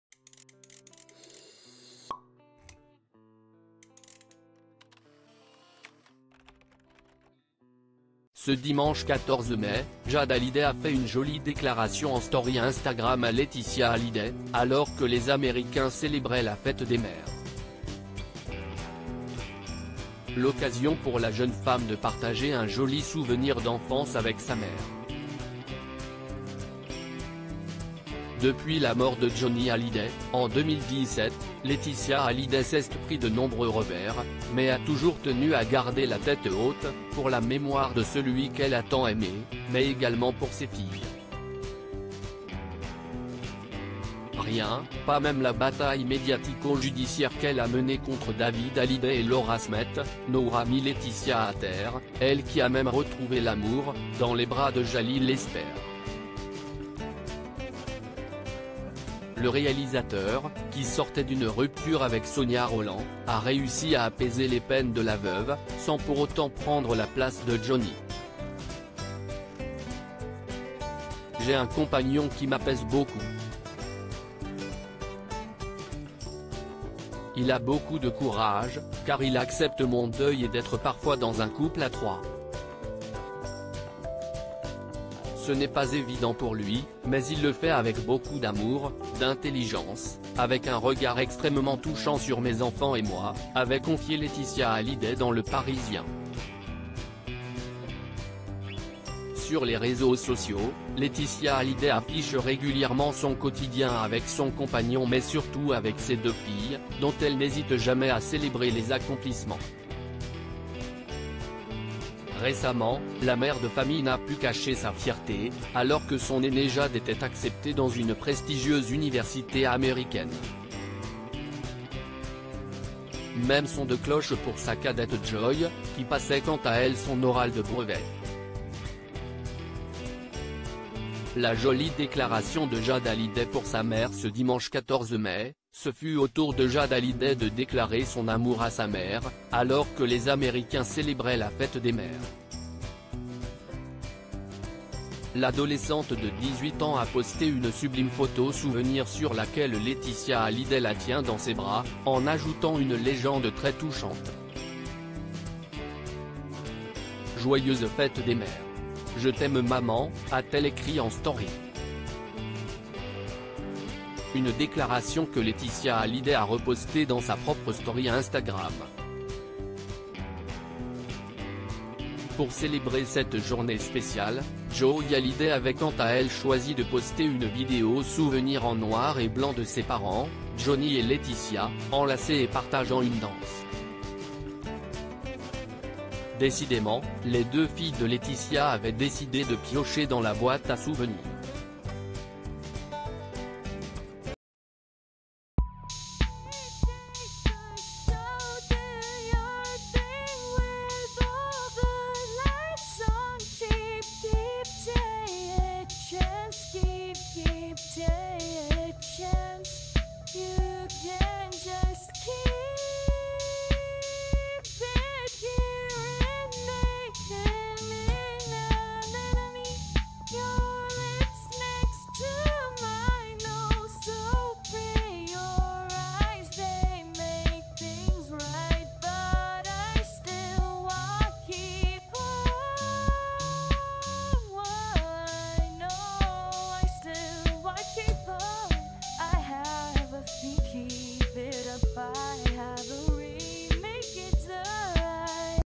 « Je t'aime » : Jade Hallyday partage un tendre souvenir avec Laeticia Hallyday pour la Fête des mèr
« Je t'aime » : Jade Hallyday partage un tendre souvenir avec Laeticia Hallyday pour la Fête des mères